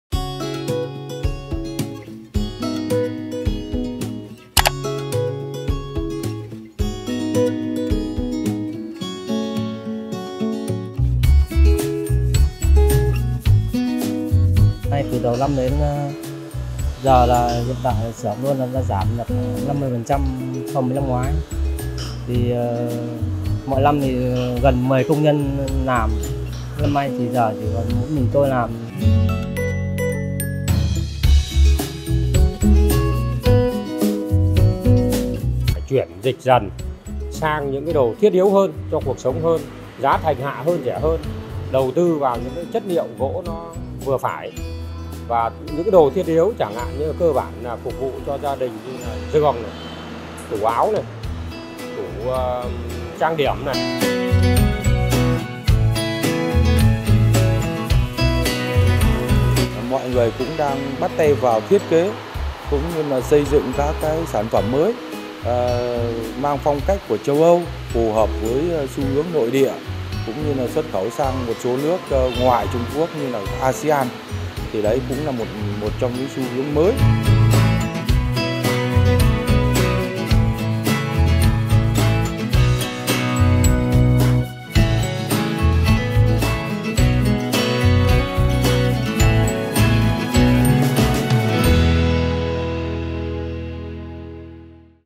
A new path
A decline in demand has left many woodwork shops in the red, forcing them to either downsize or close. But there is hope. Some firms have chosen to produce cheaper items and diversify their products for the international market.